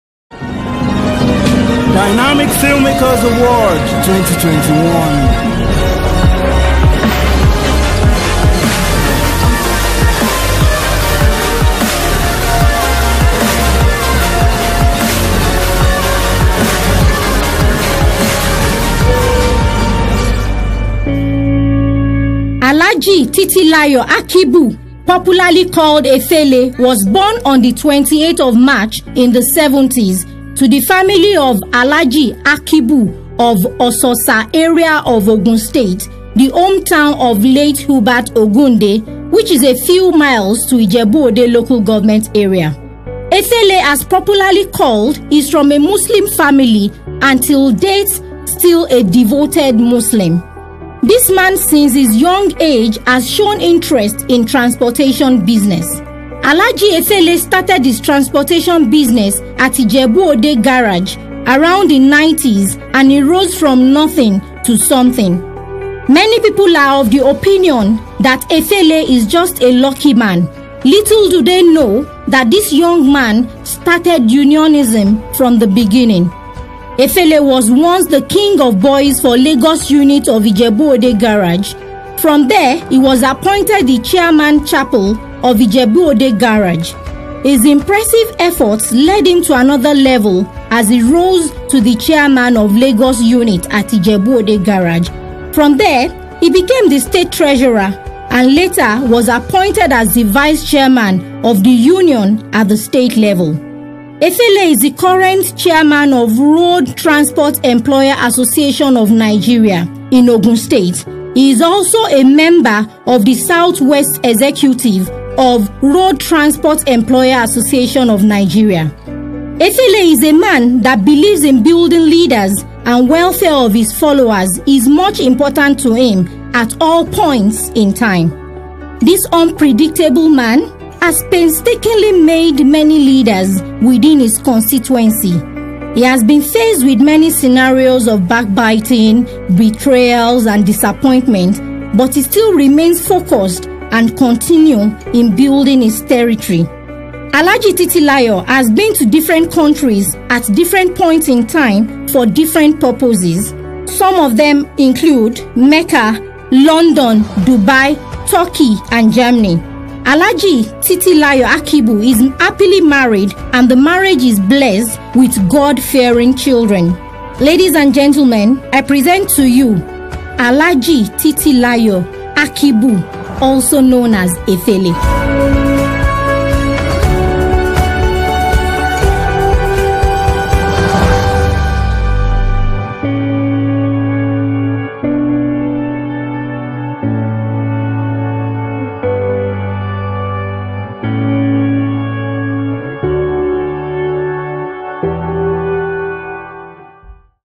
The CITATION of Alhaji Titilayo Akibu Efele
The story of a man who rose from NOTHING TO SOMETHING, FROM ZERO TO HERO! He is popularly called Efele but his real name is Titilayo